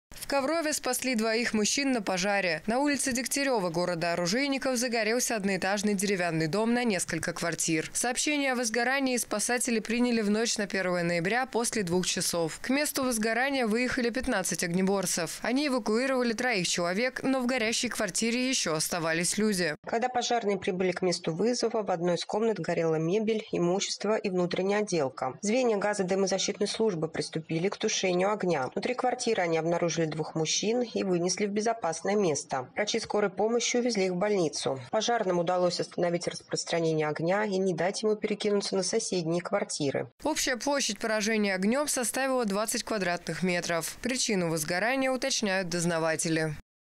На пожаре в Коврове спасли двух человек
На улице Дегтярева города оружейников загорелся одноэтажный деревянный дом на несколько квартир. К месту возгорания выехали 15 огнеборцев. Они эвакуировали троих человек, но в горящей квартире еще оставались люди.